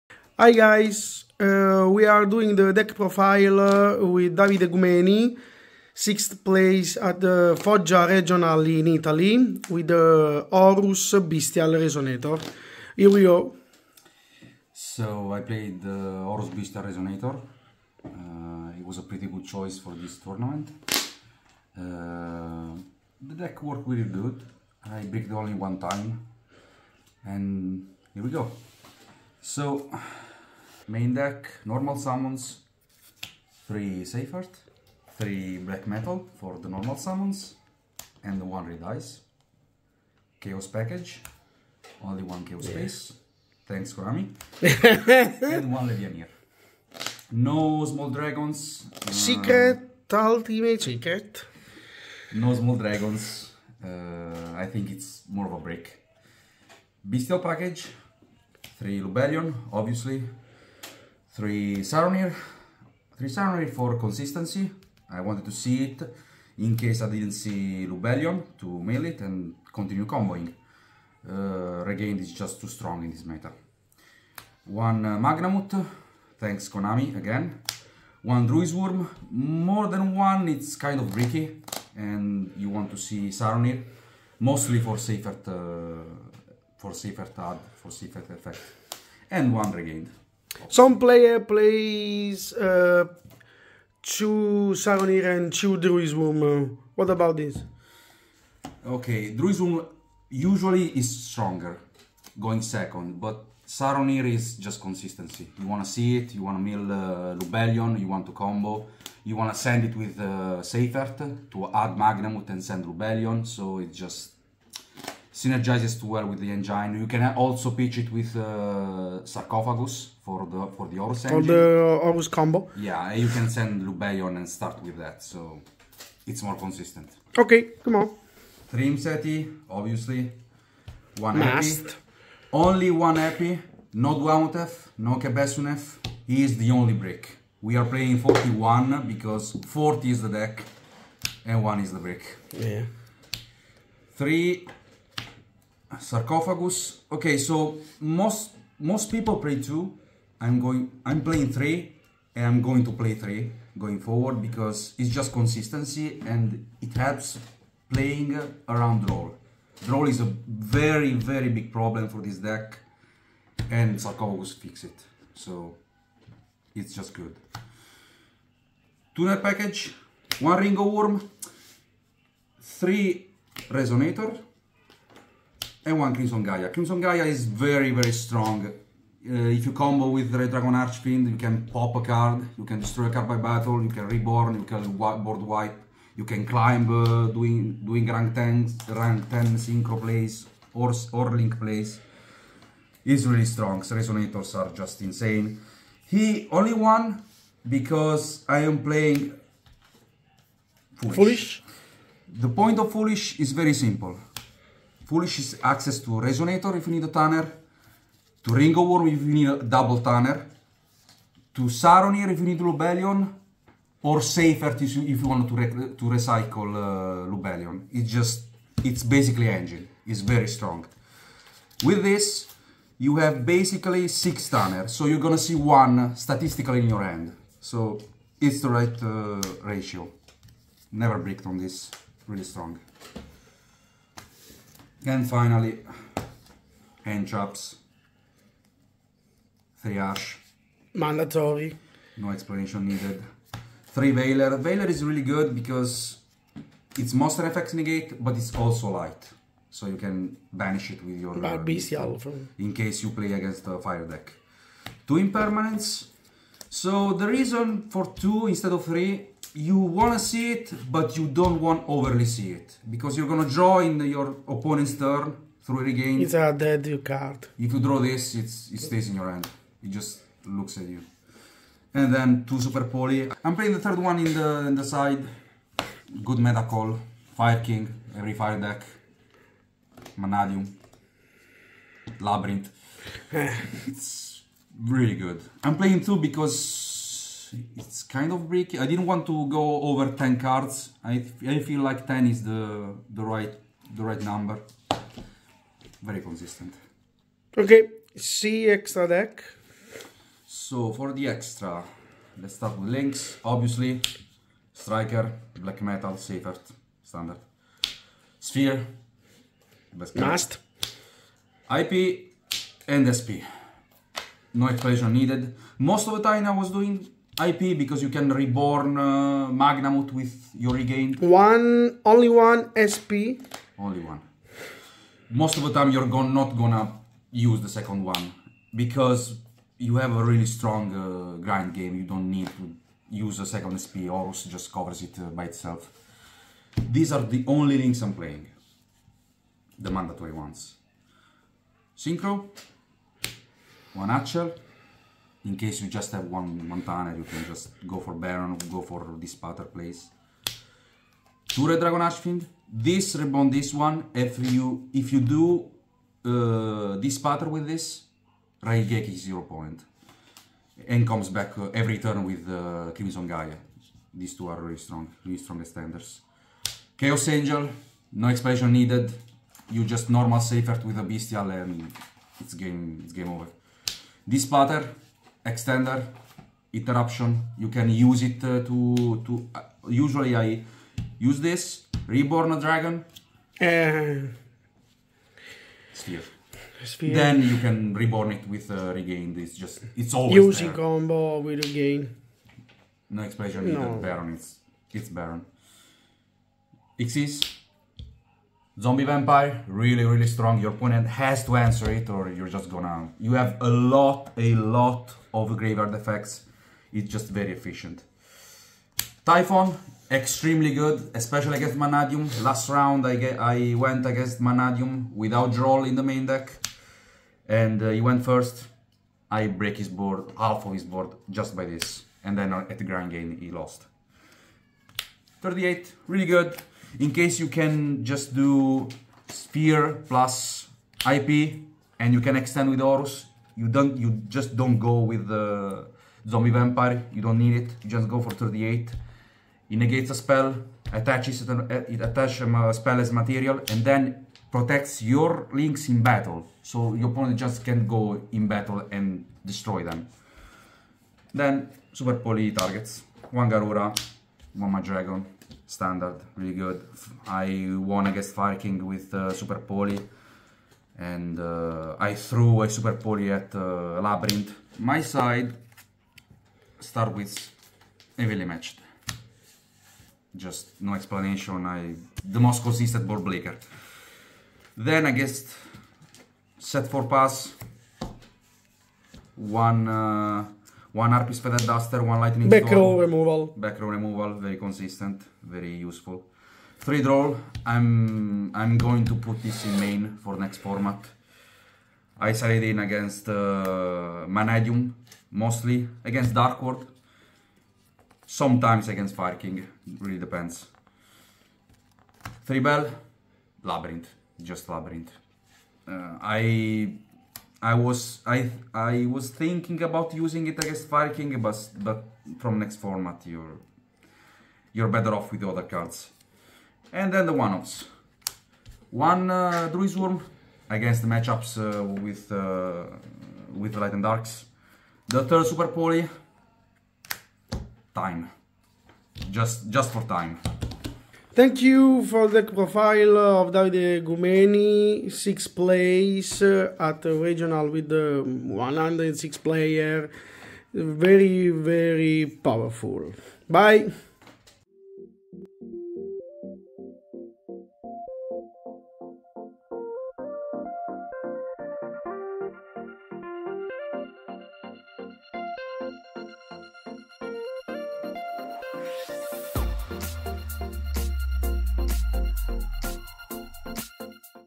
TOP 8 Foggia Italy Regionals Bystial RESONATOR Horus Deck Profile ft. Davide! | January 2024
Make sure to check out Dragonrider Games for a variety of competitive online Yu-Gi-Oh tournaments!

Davide Gumeni was able to finish Top 8 at the Foggia Regionals (Jan 13, 2024) with Bystial Resonator (Red Dragon Archfiend) featuring the Horus and Dragon Link packages! He shares his deck list and how the cards work together.

Intro: 0:00
Main Deck: 0:28
Extra Deck: 4:59
Side Deck: 7:47
Outro: 8:50